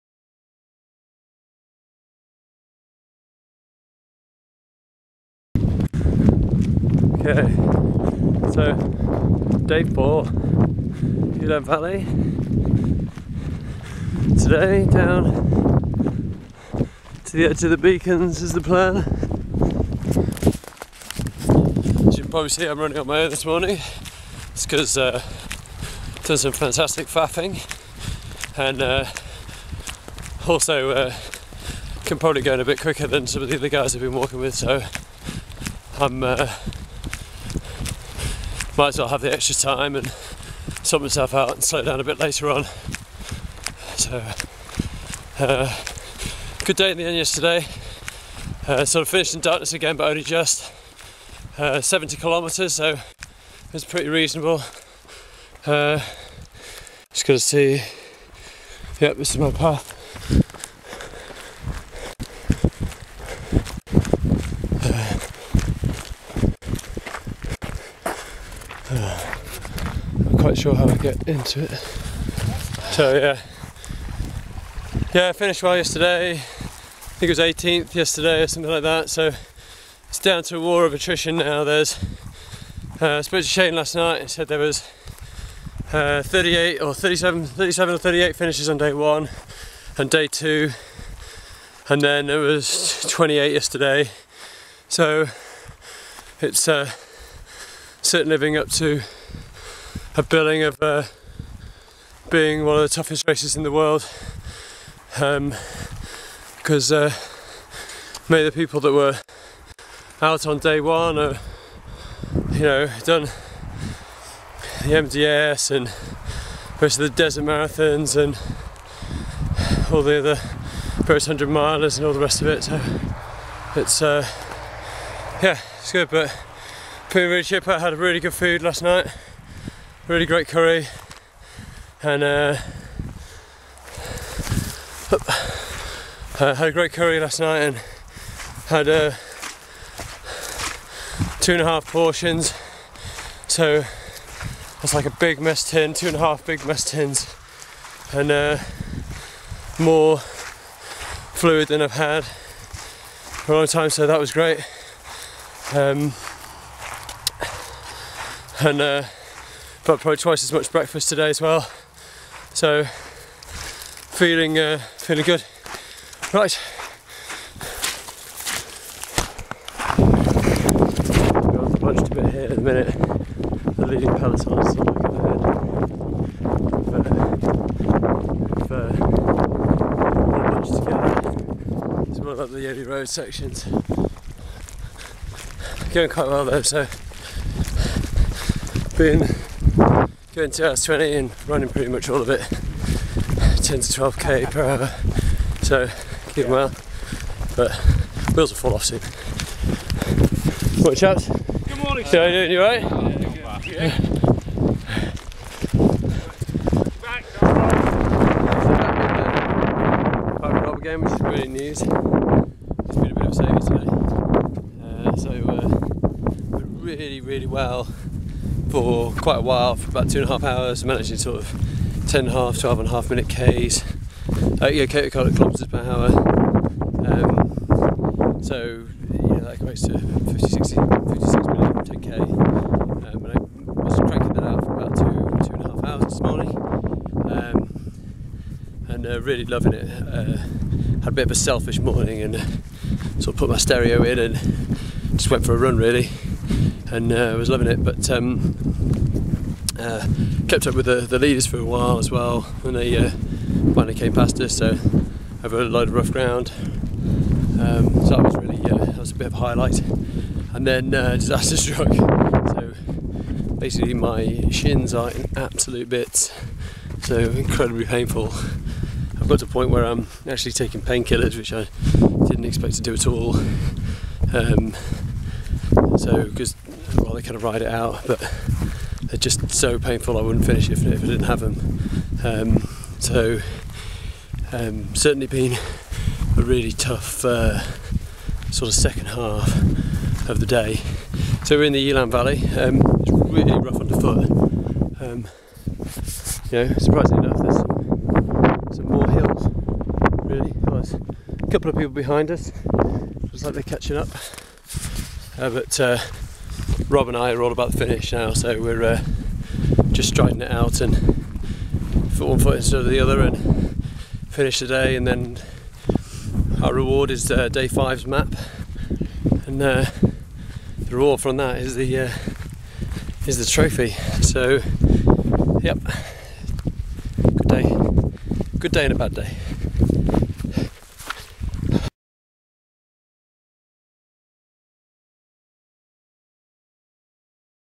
DBR Day 4 Video Diary.wmv
All my videos from the day stuck together without edit. Day 4 took in the Elan valley.